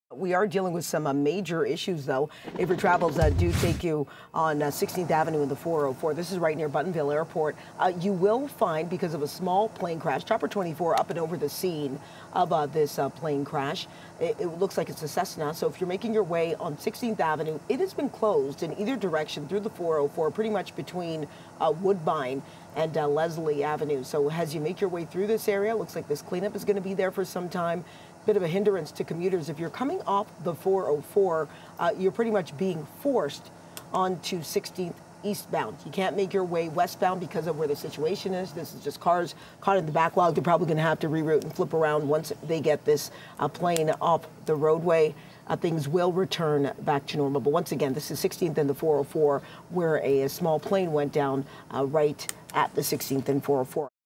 Small plane makes crash landing on the 404
Chopper 24 was above the scene on the 404 at 16th Avenue in Markham. The small plane is still largely intact. It is unclear how many people were inside.